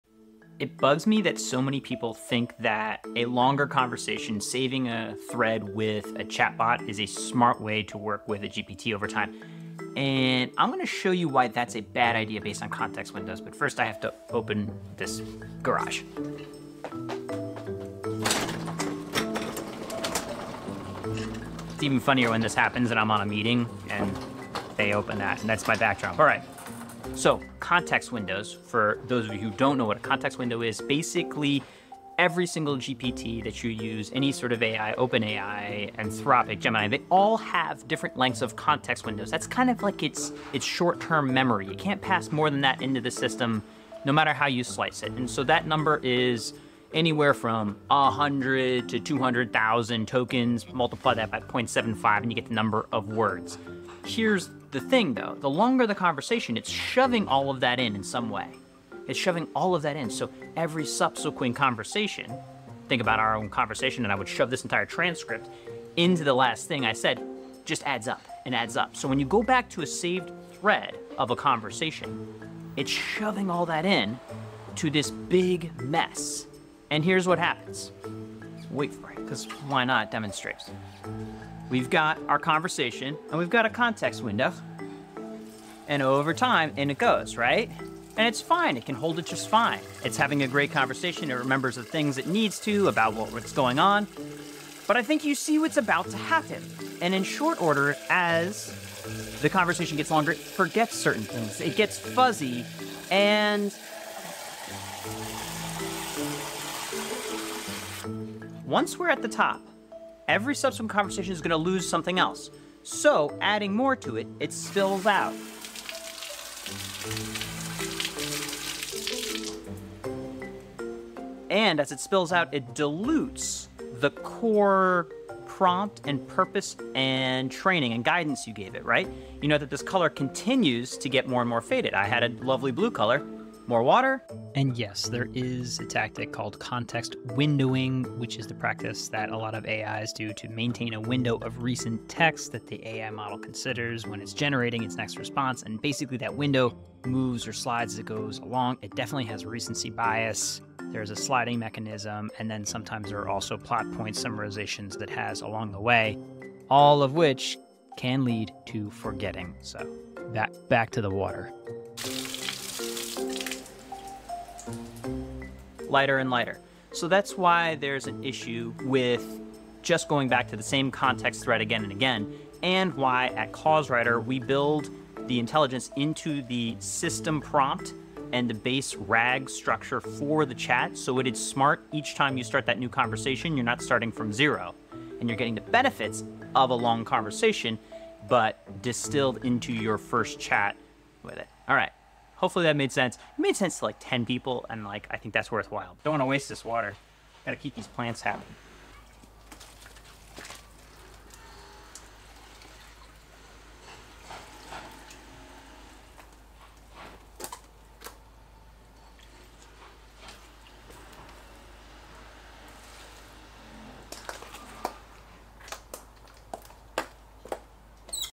The Problem With Context Windows (Why AI Fails you)
Context Windows: Why Your AI Chat is Forgetting You
"Discover why saving long AI chat threads is a flawed strategy. Learn about context windows, their impact on AI memory, and how to optimize your interactions with GPTs for better results. Unlock the secrets to smarter AI conversations and avoid common pitfalls in just a few minutes.

CauseWriter.ai - Custom AI built for your brand.

WholeWhale.com - B Corp digital agency - ads, analytics, SEO, Content Marketing, Digital fundraising.